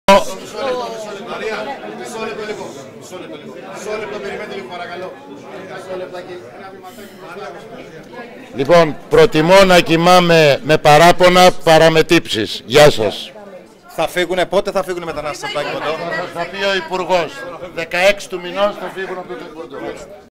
Δήλωση δημάρχου Παλαιού Φαλήρου
Τι δήλωσε ο δήμαρχος Παλαιού Φαλήρου μετά τη συνάντηση της ΚΕΔΕ με τον υπουργό Μετανάστευσης.